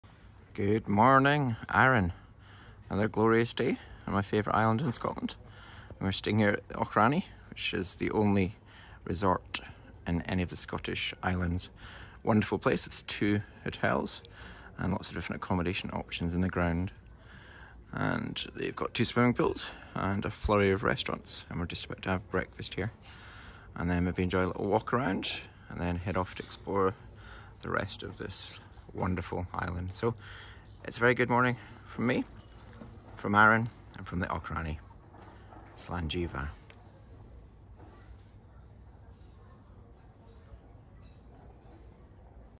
Auchrannie Resort, Isle of Arran, Scotland
Spectacular mountain views from Cruize bar and brasserie at the Auchrannie Resort on the Isle of Arran.